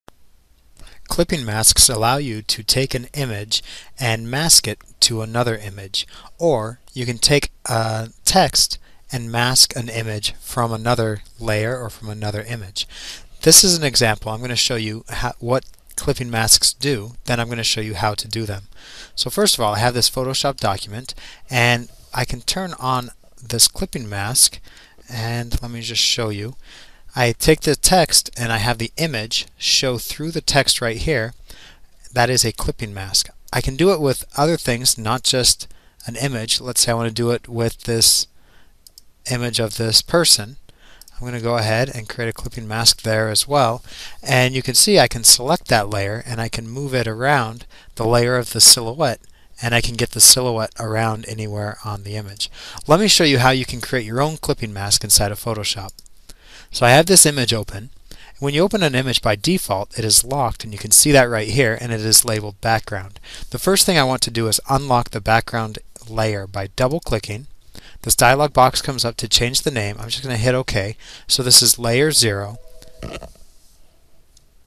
Photoshop Tutorial: Fill text with an image using clipping mask
Learn how to use a clipping mask to put a picture inside of text or another picture.